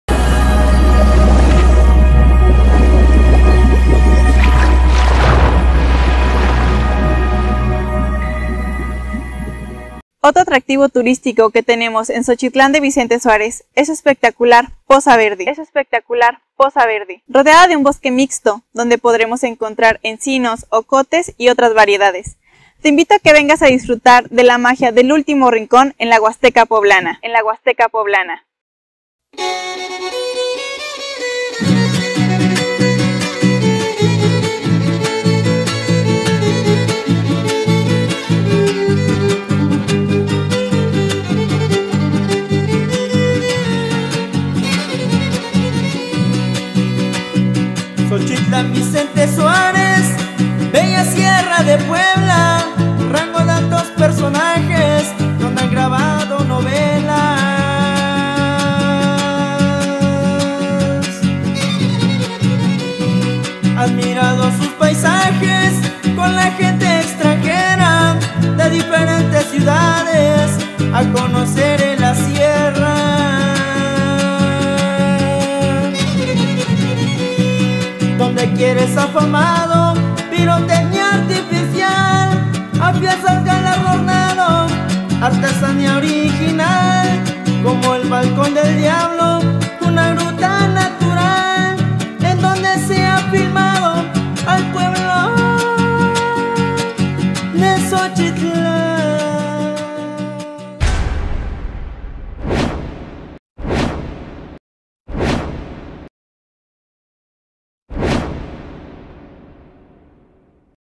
VISITA LA POZA VERDE
IRENE LUNA GODOZ MISS TURISMO PUEBLOS MAGICOS MEXICO TE INVITA A VIVIR ESTA MARAVILLA NATURAL UBICADA EN XOCHITLAN DE VICENTE SUAREZ, PUEBLA.